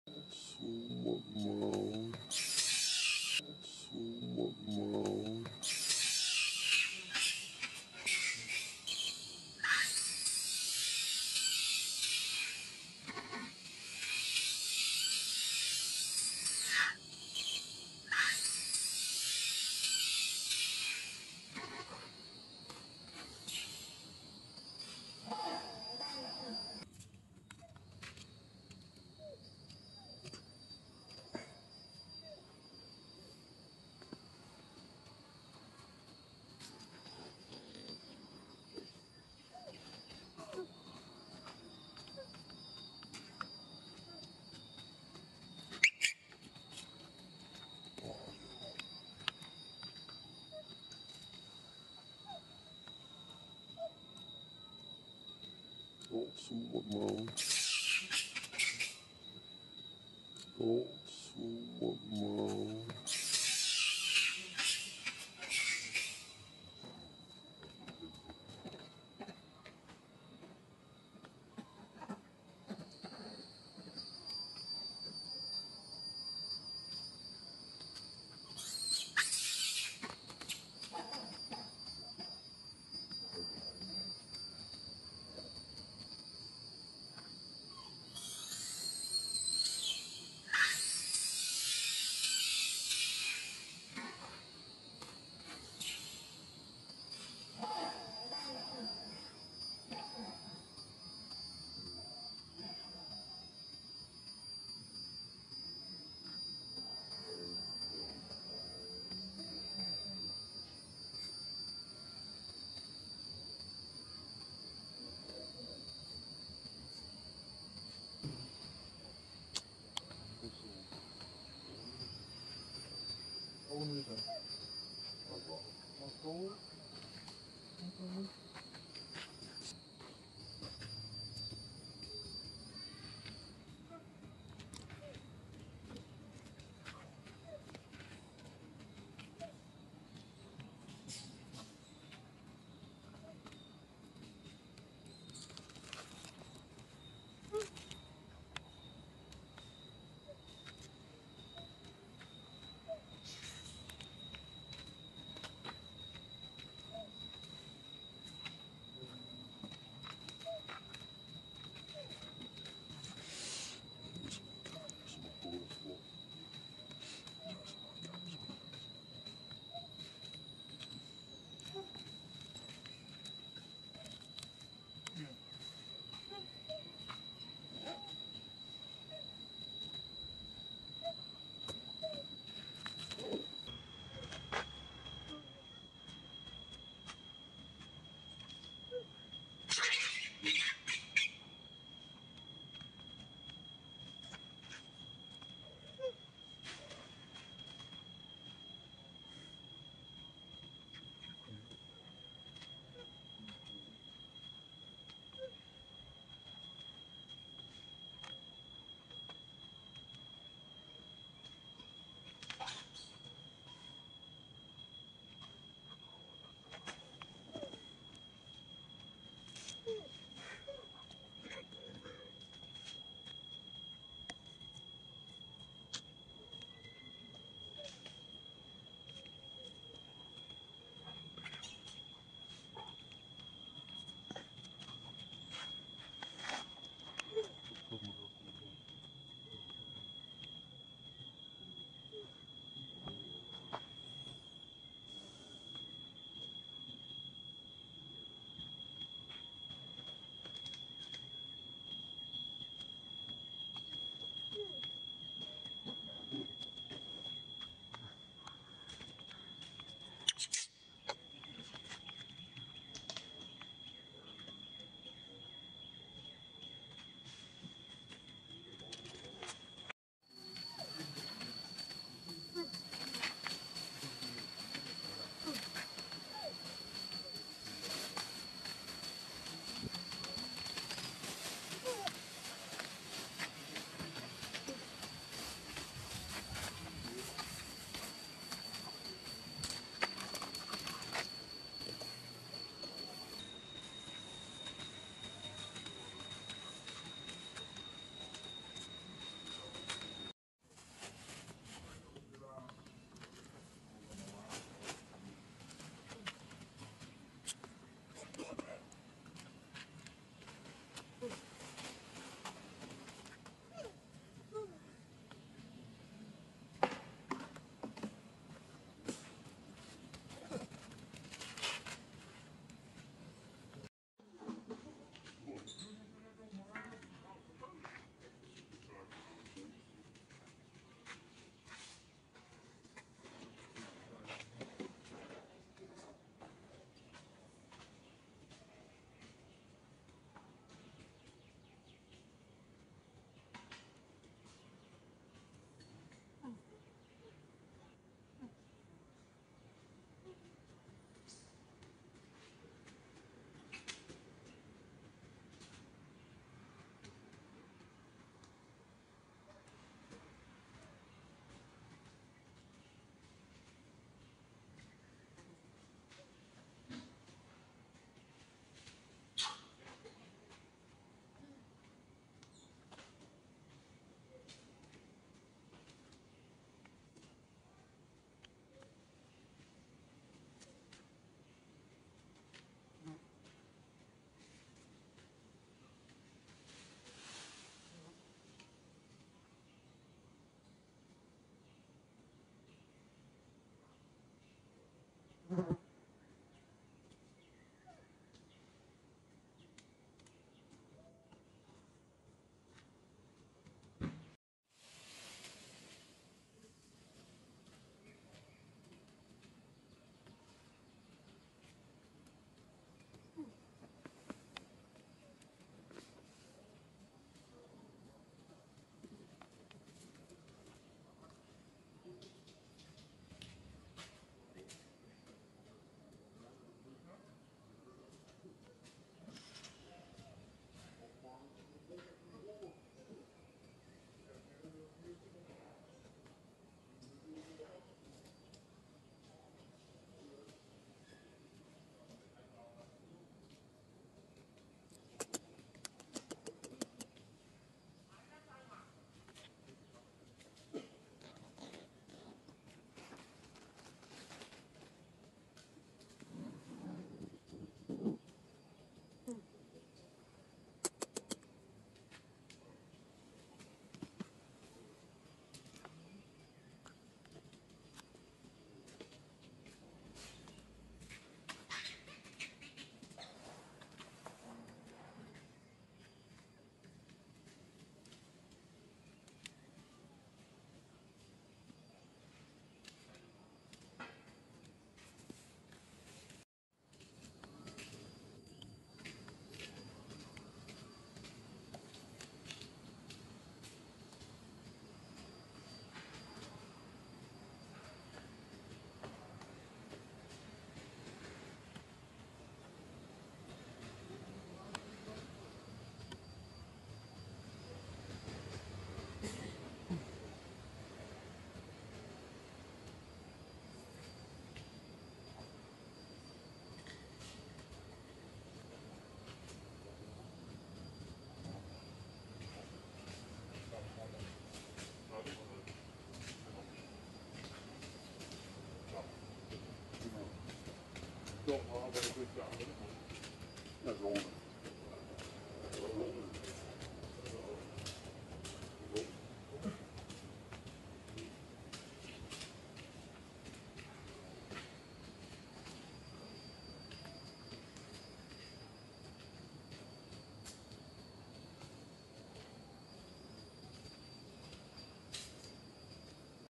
Update: Sweetie MOKA feels not happy with her Little Brother, Mora, He always grabs her food so She.
Video Today: Update: Sweetie MOKA feels not happy with her Little Brother, Mora, He always grabs her food so She.

Best Regards,